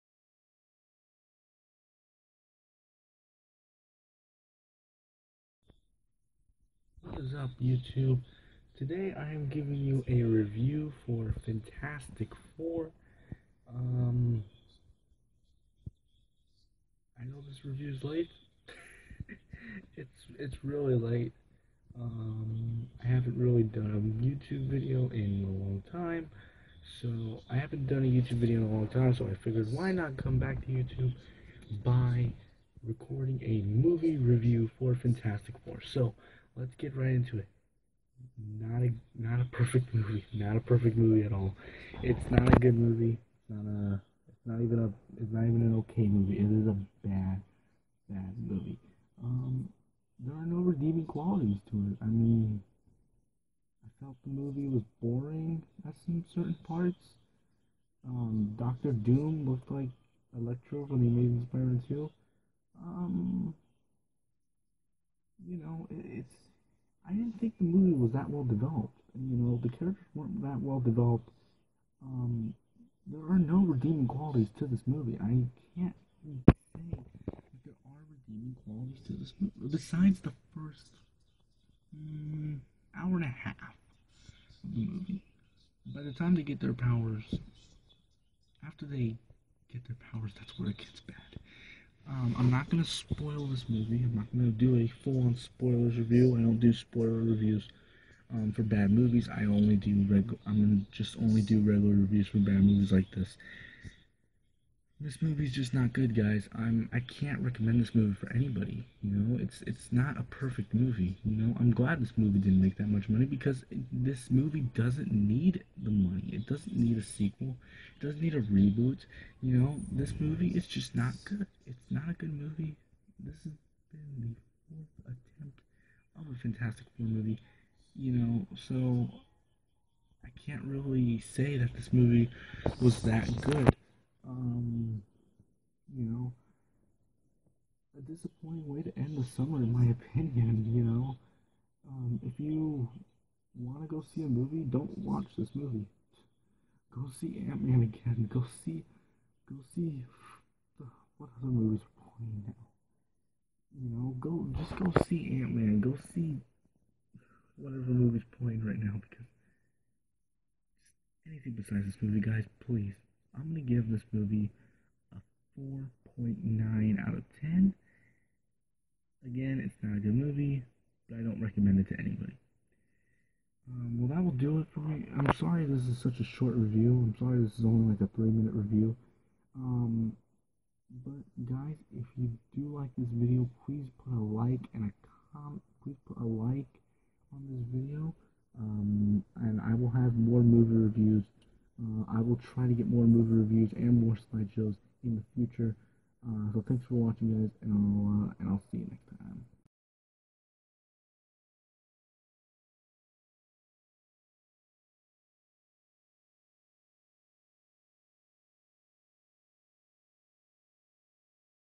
Fantastic 4 review
Zeke Addington does his first movie review for the disappointment that is Fant4stic.